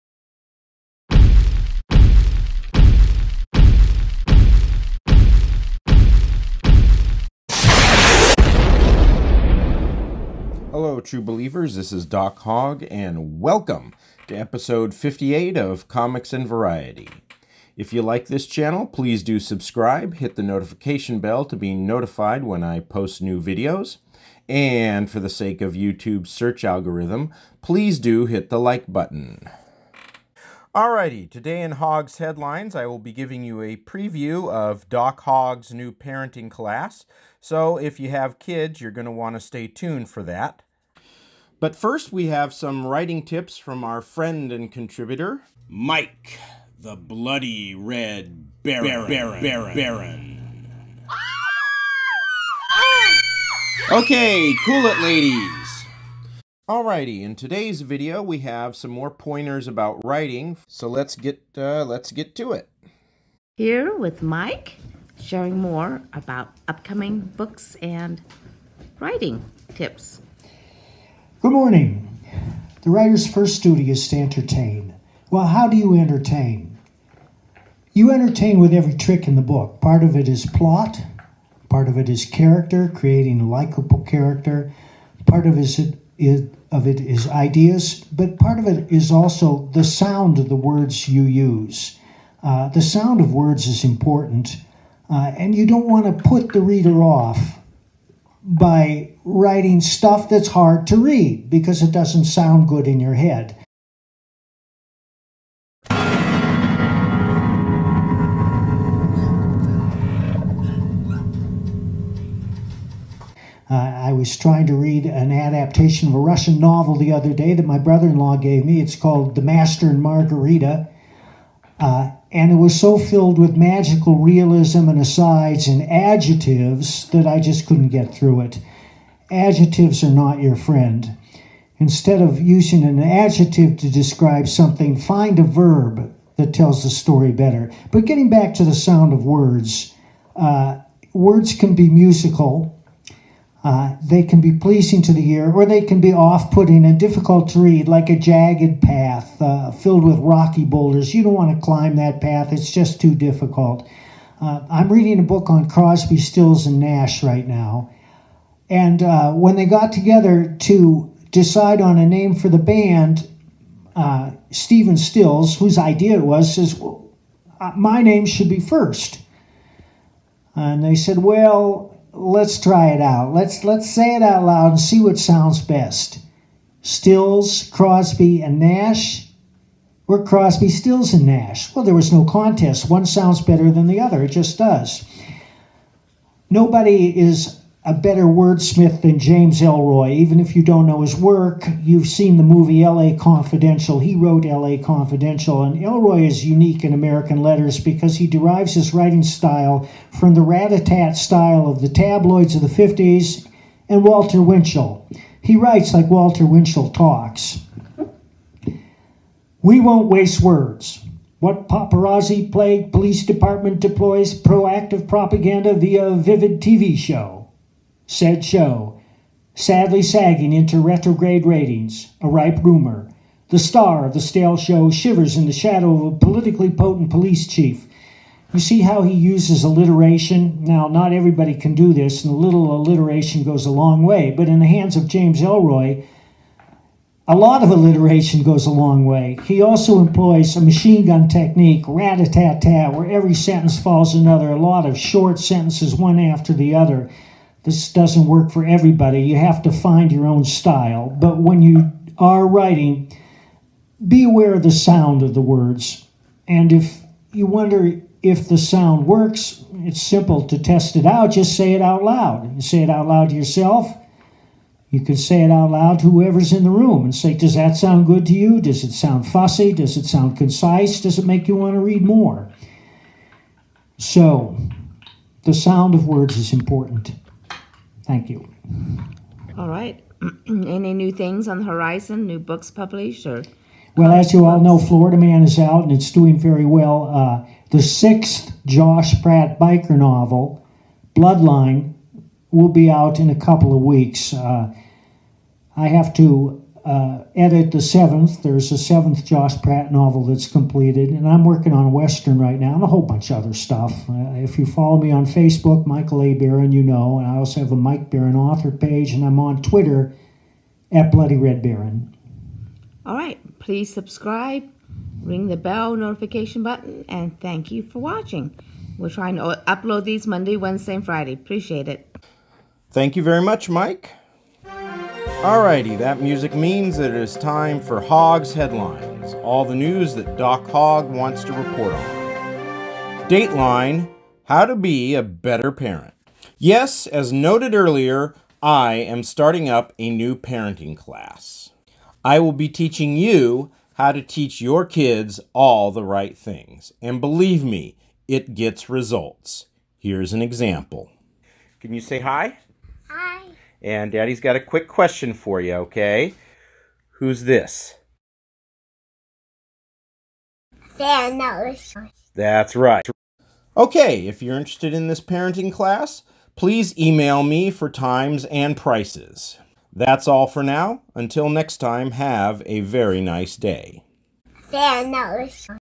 How To Keep The Reader Entertained | How To Be A Better Parent
Comic book writer Mike Baron explains how to keep the reader entertained, with a focus on how to make words sound pleasing.  And in Hog's Headlines: Doc Hog Explains How To Use Thanos To Be A Better Parent!